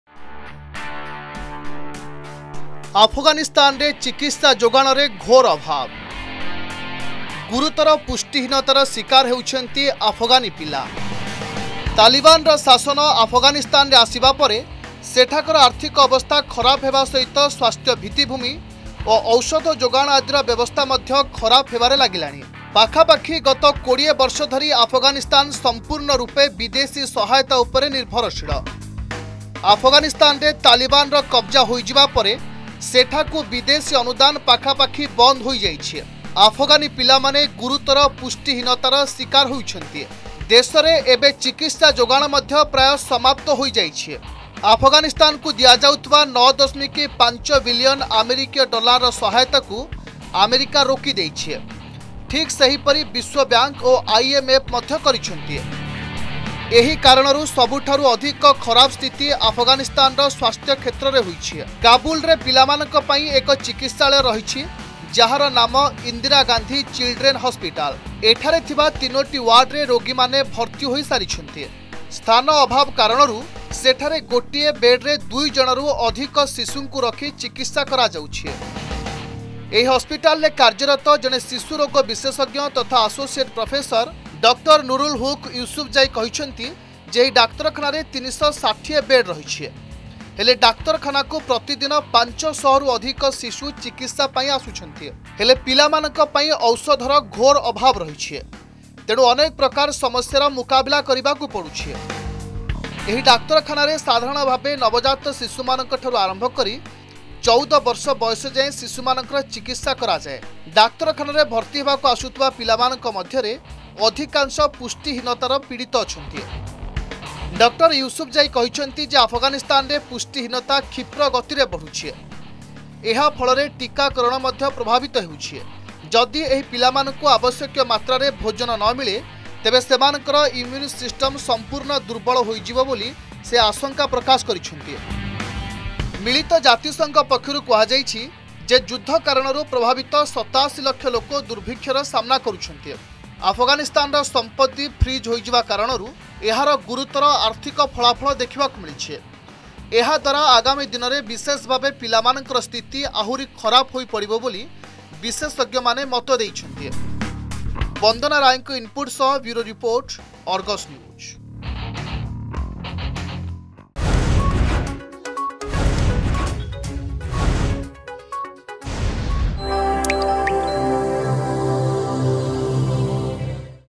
ଗୁରୁତର ପୁଷ୍ଟିହୀନତାର ଶୀକାର ଆଫଗାନୀ ଶିଶୁ
ଆଫଗାନିସ୍ତାନରେ ଚିକିତ୍ସା ଯୋଗାଣରେ ଘୋର ଅଭାବ । ଗୁରୁତର ପୁଷ୍ଟିହୀନତାର ଶୀକାର ହେଉଛନ୍ତି ଆଫଗାନୀ ପିଲା । ତାଲିବାନର ଶାସନ ଆଫଗାନିସ୍ତାନରେ ଆସିବା ପରେ ସେଠାକାର ଆର୍ଥିକ ଅବସ୍ଥା ଖରାପ ହେବା ସହିତ ସ୍ୱାସ୍ଥ୍ୟ ଭିତିଭୂମି ଓ ଔଷଧ ଯୋଗାଣ ଆଦିର ବ୍ୟବସ୍ଥା ମଧ୍ୟ ଖରାପ ହେବାରେ ଲାଗିଲାଣି ।